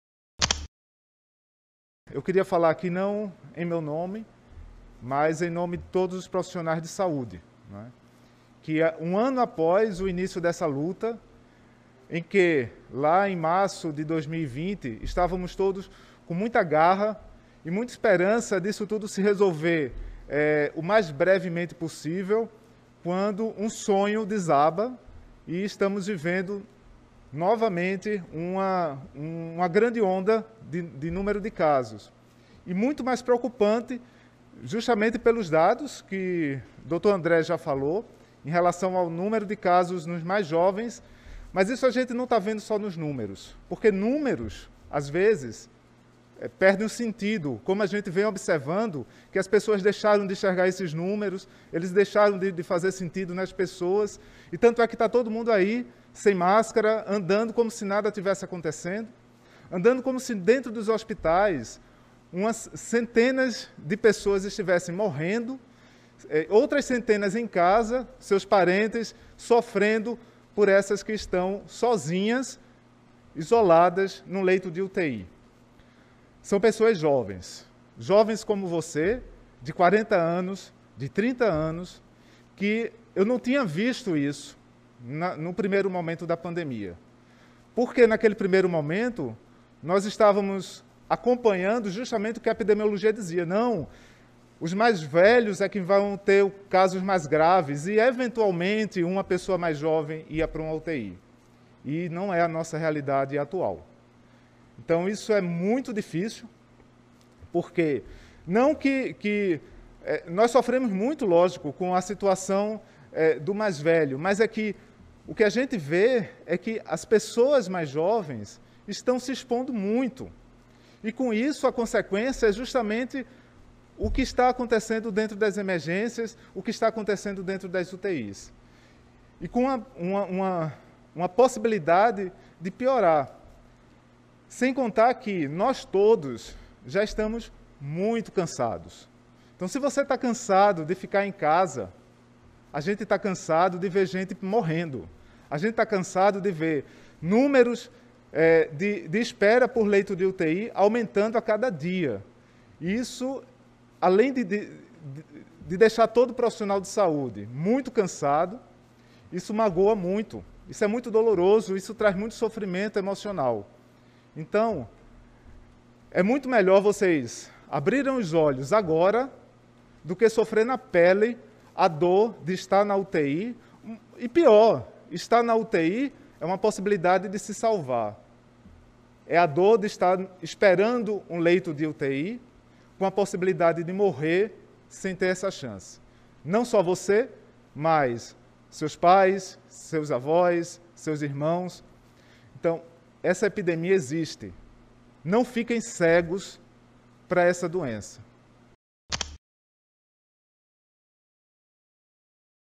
Covid-19: Infectologista faz apelo a população e desabafa sobre a situação dos hospitais
Em coletiva de imprensa nesta quinta-feira (25), em que foi anunciada a extensão da quarentena mais rígida até o próximo dia 31 de março em Pernambuco, o médico Demetrius Montenegro, chefe do setor de Infectologia do Hospital Universitário Oswaldo Cruz (Huoc), frisou o quanto tem se preocupado com a frequência de adultos jovens que chegam aos hospitais com sintomas de covid-19. Nesta quinta (25) o Estado ultrapassa a marca de 1.800 pessoas que recebem atualmente assistência em terapia intensiva (UTI). Entre elas, 1.424 estão em leitos públicos e 418 em vagas privadas.

Saiba mais no JC.COM.BR